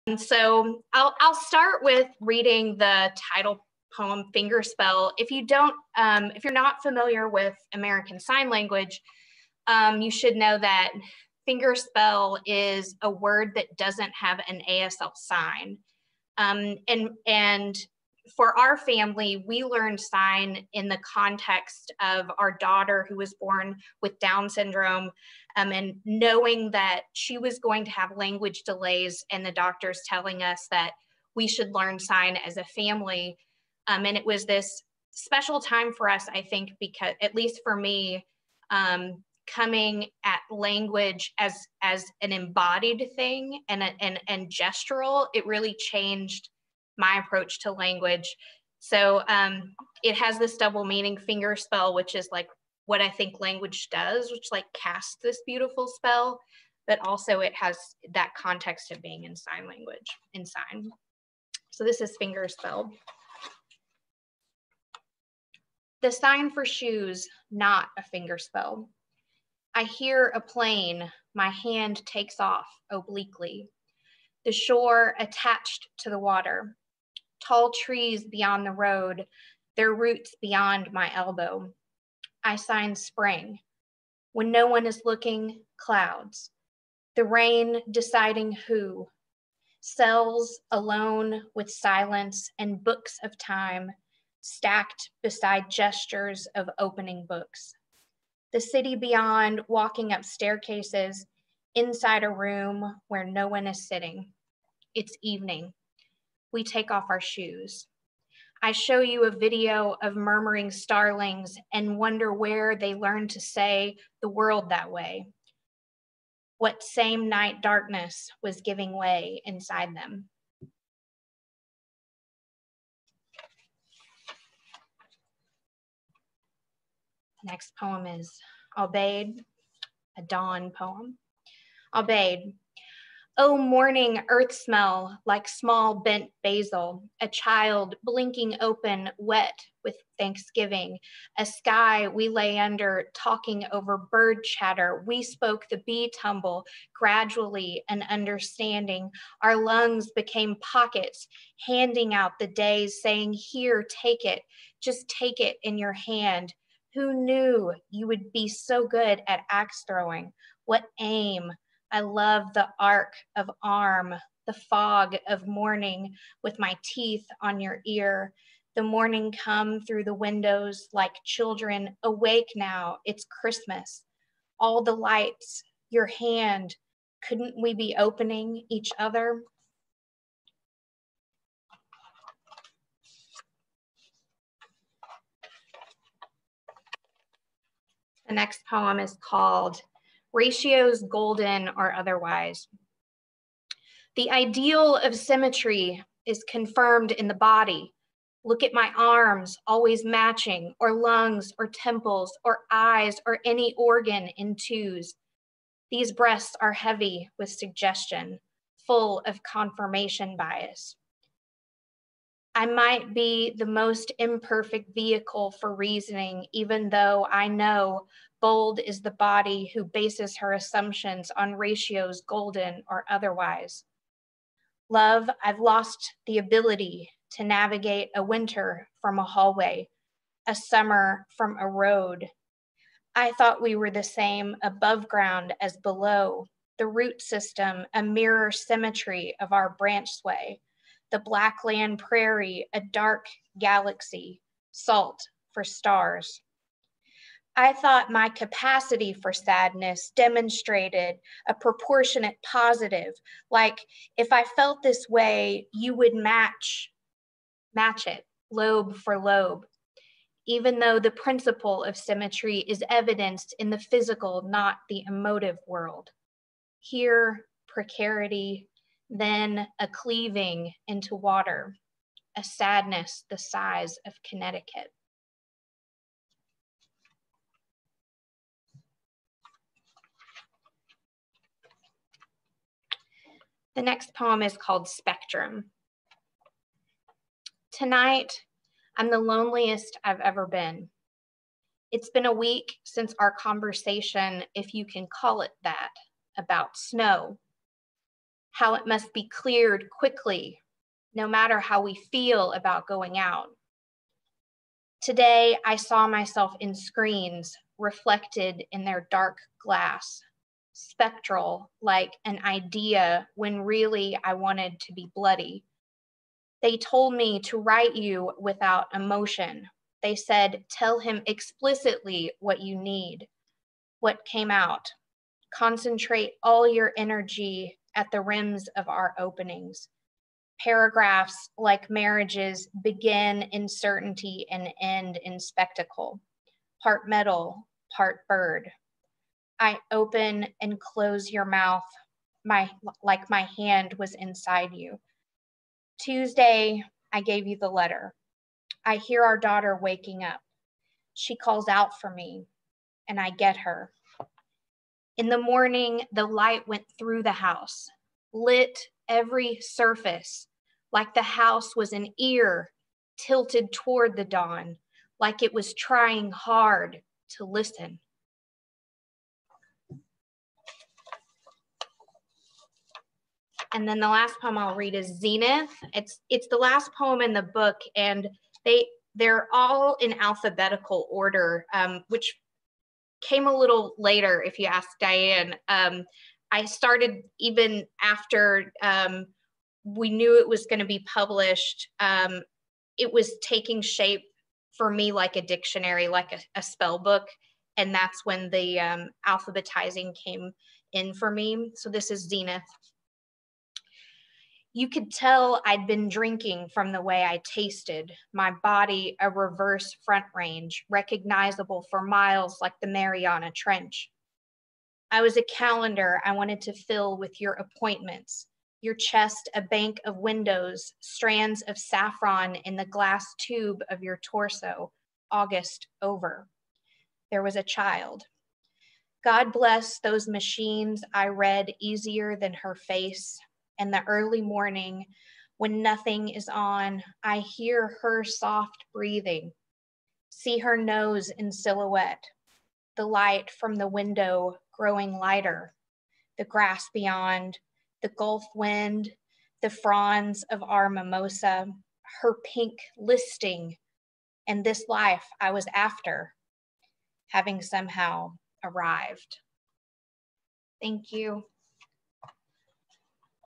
Black Lawrence Press Spring '21 Virtual Reading Series Presents: Lindsay Illich
Lindsay Illich reads from her latest poetry collection, Fingerspell.

Lindsay Illich is the author of Fingerspell (Black Lawrence Press, 2020) and Rile & Heave (Texas Review Press, 2017). Rile & Heave won the Texas Review Press Breakthrough Prize in Poetry. She also co-authored Teach Living Poets (NCTE, 2021) with Melissa Alter Smith, a book that introduces secondary English teachers to the flourishing world of contemporary American poetry. Currently, Lindsay directs the Writing Program at Curry College in Milton, Massachusetts.